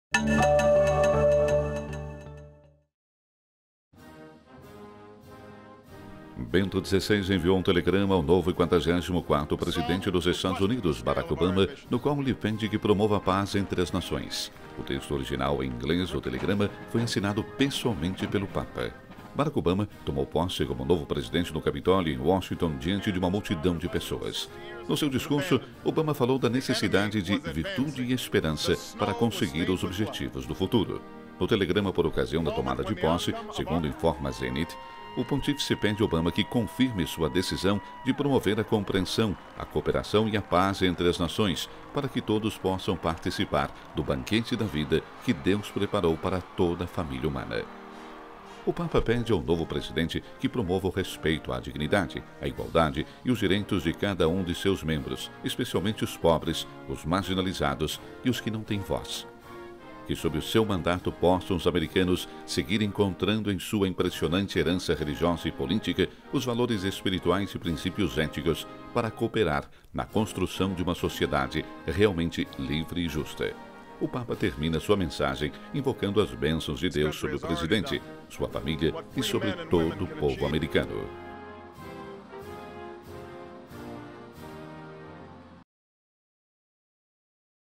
O Papa pede a Obama que promova a paz
Bento XVI enviou um telegrama ao novo e quadragésimo quarto presidente dos Estados Unidos, Barack Obama, no qual lhe pede que promova a paz entre as nações. O texto original em inglês do telegrama foi assinado pessoalmente pelo Papa.Barack Obama tomou posse como novo presidente no Capitólio em Washington diante de uma multidão de pessoas. No seu discurso Obama falou da necessidade de virtude e esperança para conseguir os objetivos do futuro. No telegrama por ocasião da tomada de posse, segundo informa Zenit, o Pontífice pede a Obama que "confirme sua decisão de promover a compreensão, a cooperação e a paz entre as nações, para que todos possam participar no banquete da vida que Deus preparou para toda a família humana". O Papa pede ao novo presidente que promova "o respeito à dignidade, a igualdade e os direitos de cada um de seus membros, especialmente os pobres, os marginados e os que não tem voz". "Que sob o seu mandato possam os americanos seguir encontrando em sua impressionante herança religiosa e política os valores espirituais e princípios éticos para cooperar na construção de uma sociedade realmente livre e justa". O Papa termina sua mensagem invocando "as bênçãos de Deus sobre o presidente, sua família e sobre todo o povo americano".